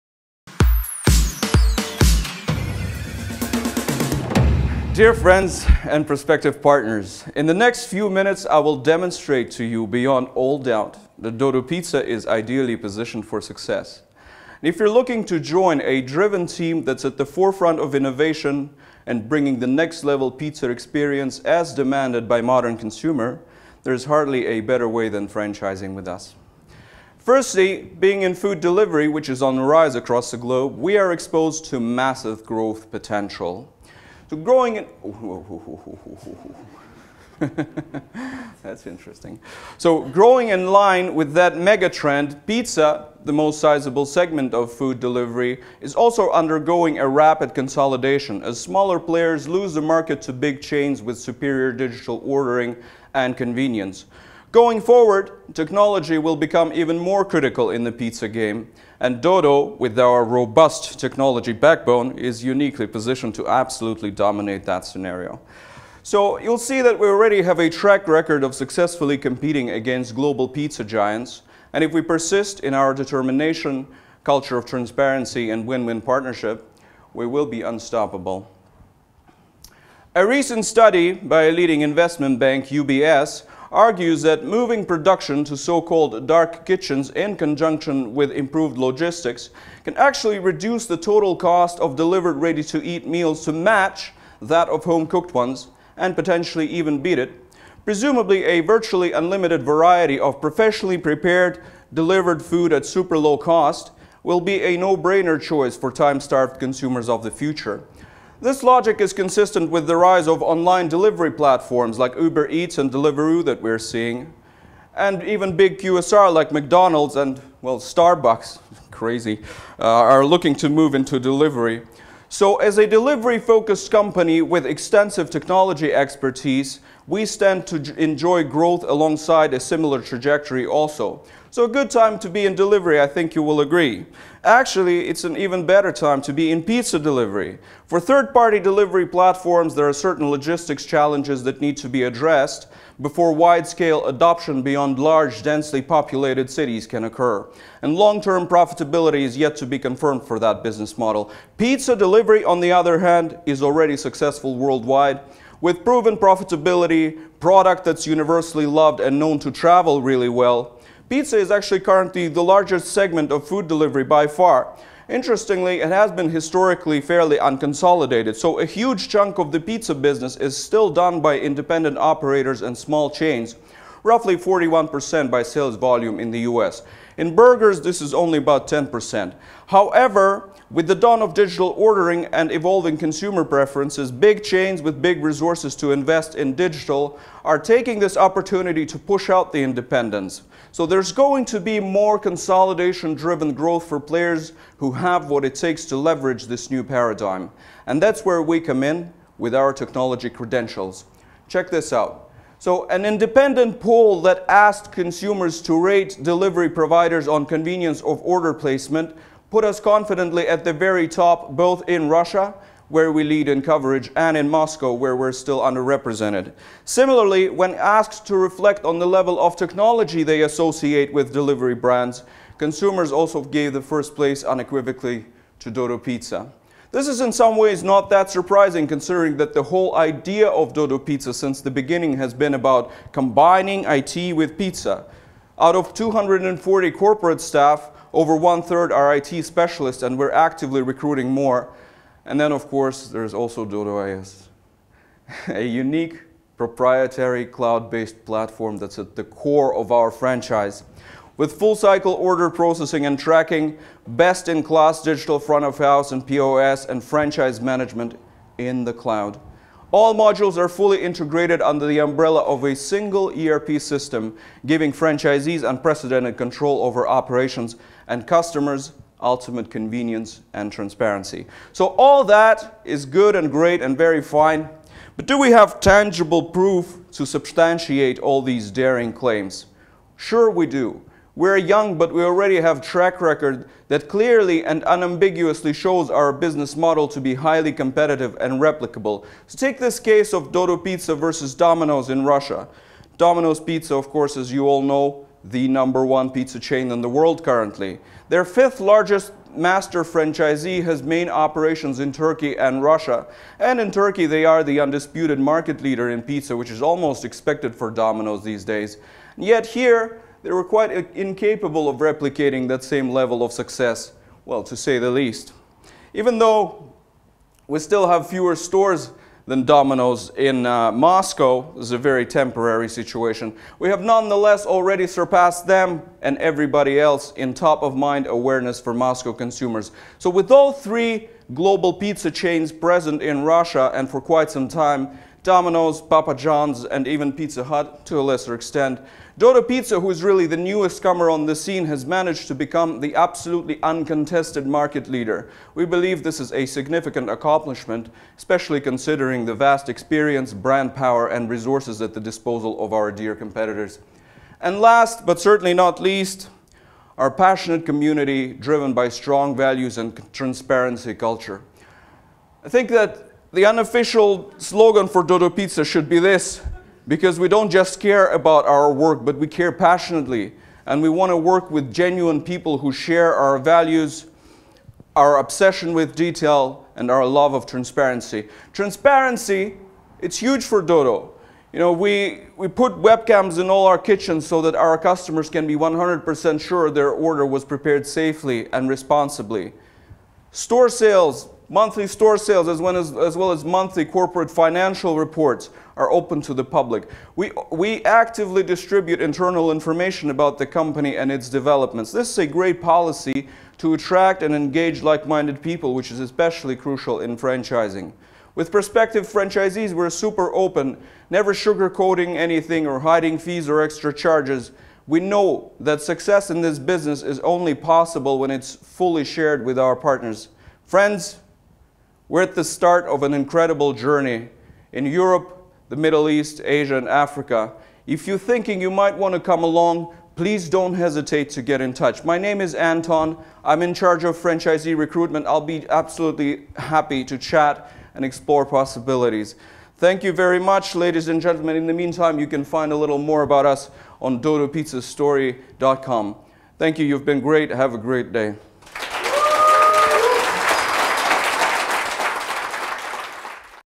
5 reasons to be a Dodo franchisee. Anton Deryagin. February 4, 2019
Learn more about the Dodo franchise, read our Manifesto:
Why people hate franchising and how we dare to change that